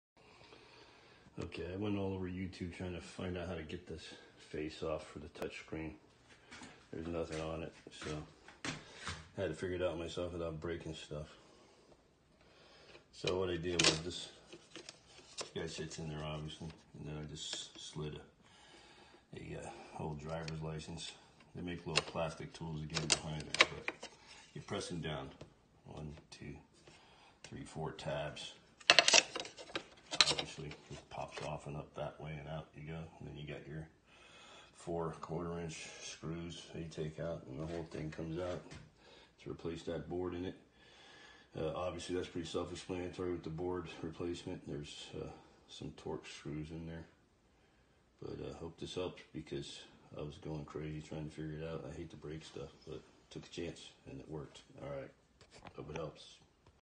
CYE22TSHESS GE profile Cafe dispenser control panel touch screen WR55X30439
How to remove dispenser buttons cover.